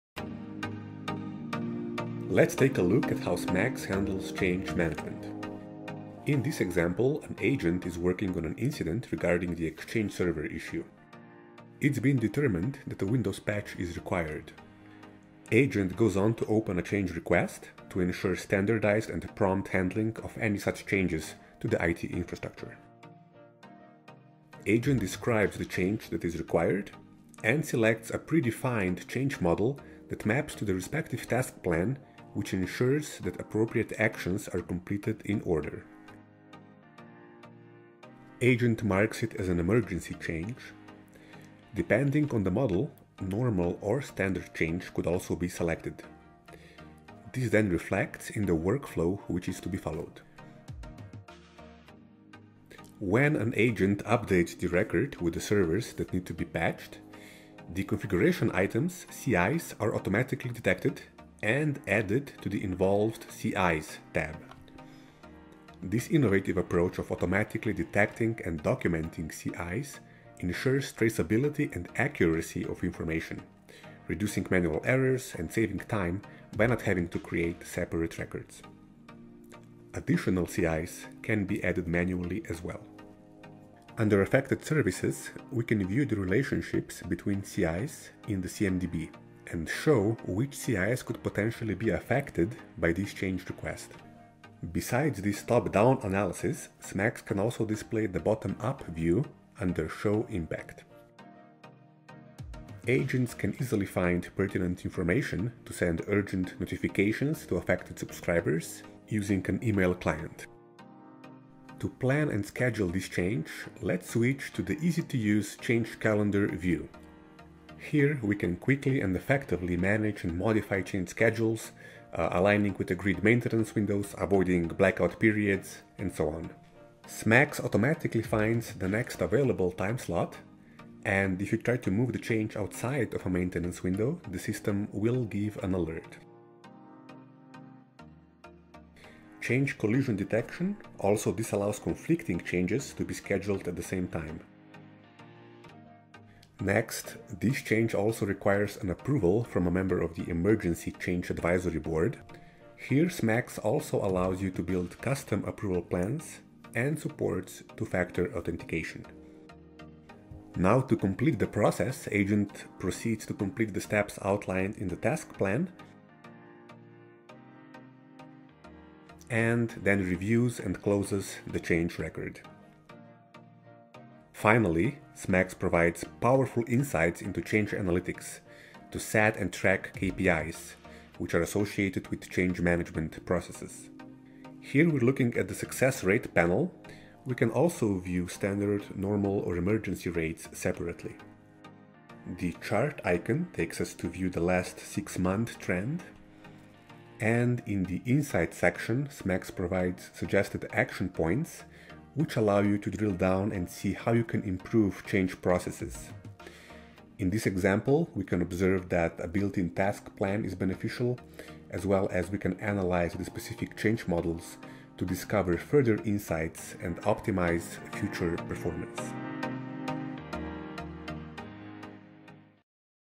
Change and Release Management
- In this video, we walk through how IT service management solution SMAX, handles change management. You'll learn how to create a change request, send information to clients, see your KPI insights and more.